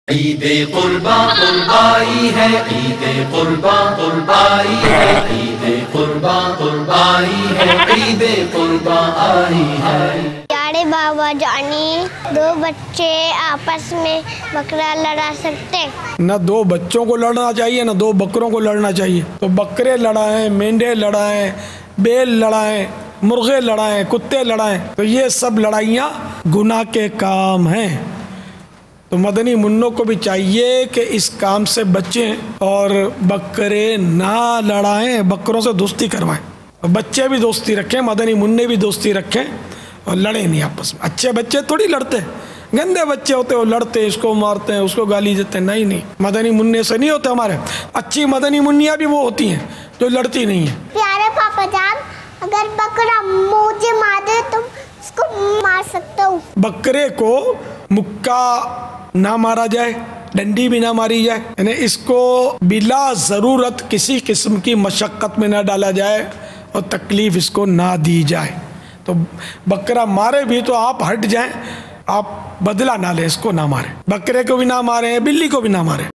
Bakra Larwana - Qurbani Kay Masail - Maulana Ilyas Qadri
This Qurbani Video uniquely arranged for the direction of Muslims of the world to lead qurbani celebration 2016 containing a short bayan of Madani Muzakra on point of "Goat Fighting" (Bakra Larwana - بکرے کی لڑائی).

One of the well known system of Madani Channel on the occasion of Qurbani 2016.

We can learn and get more data and strategy to do qurbani in right way under sharia direction.

Maulana Ilyas Qadri circulated astounding valuable data as madani pearls (madani phool - مدنی پھول) in the light of Quran and Hadith.